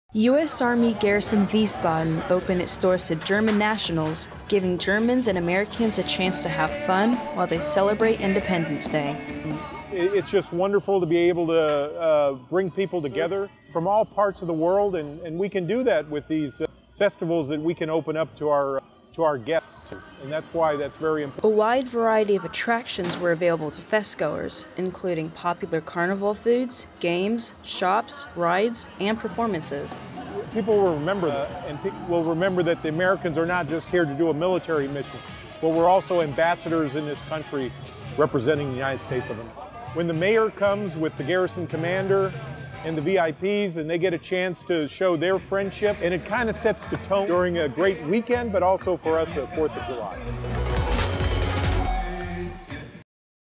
Americans and Germans come together for 5-day festival
The Wiesbaden Family and MWR Wiesbaden hosted the German-American Friendship Fest, June 30 - July 4, in the lower Hainerberg Housing area in Wiesbaden, DE. The carnival style event was open to all DoD ID card holders and German citizens residing in the local Wiesbaden community. (U.S. Army video by Pfc. Deziree Keay)